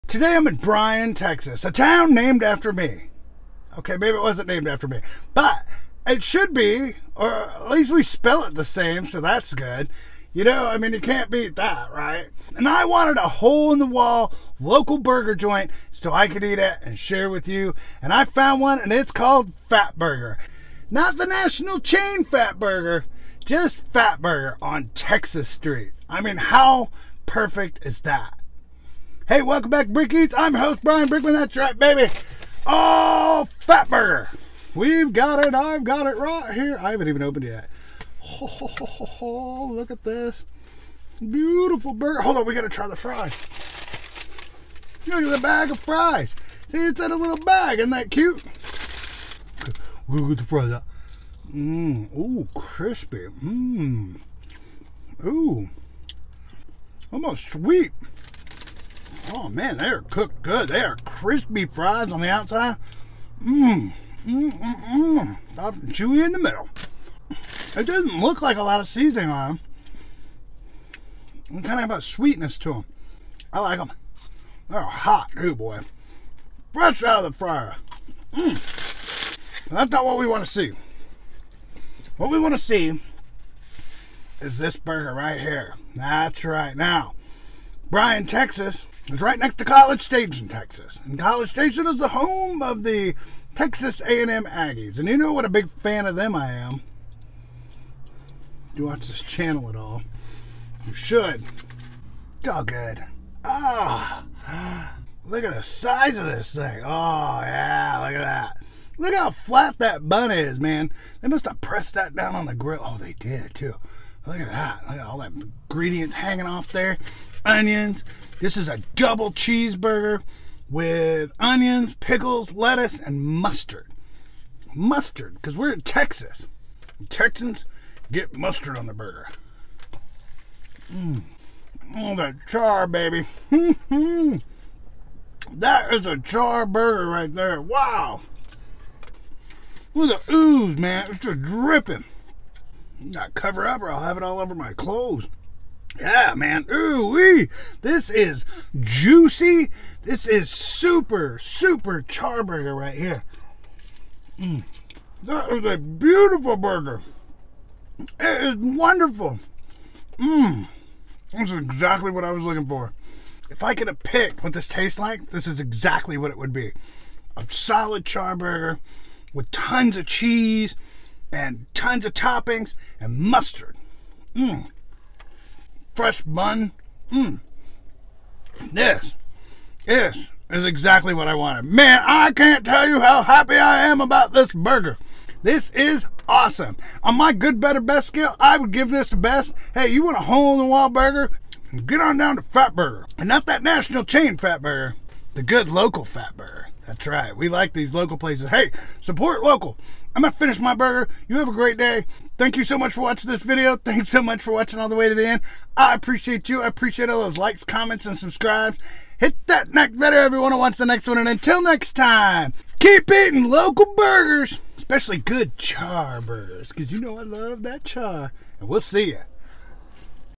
Fat Burger REVIEW in Bryan, Texas- Eat Local- brickeats
There's a burger joint in Bryan Texas called Fat Burger and it is NOT the national chain, it's a local burger joint and it was recommended to me by a former resident so I just had to try it!